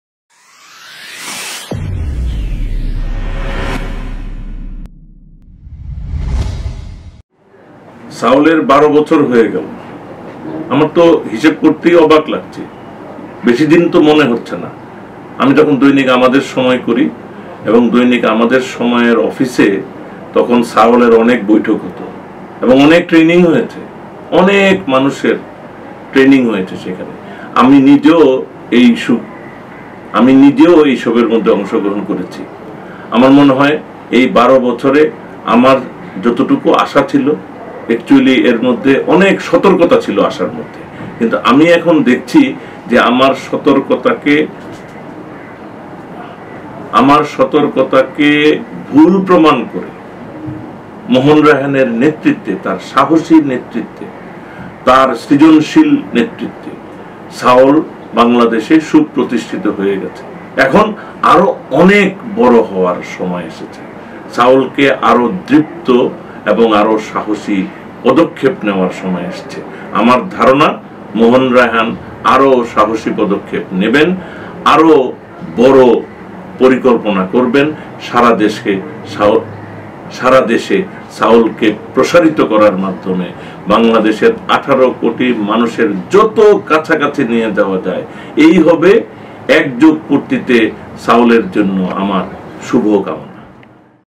সাওল হার্ট সেন্টার (বিডি) লিমিটেড- সাফ্যলের ১২ বছর পূর্তি
সাওল হার্ট সেন্টার (বিডি) লিমিটেড- সাফ্যলের ১২ বছর পূর্তি উদযাপন অনুষ্ঠানে ভিডিও বার্তায় শুভেচ্ছা জানান-
নাঈমুল ইসলাম খান
নতুনধারার সংবাদপত্রের পথিকৃৎ। সম্পাদক, আমাদের নতুন সময়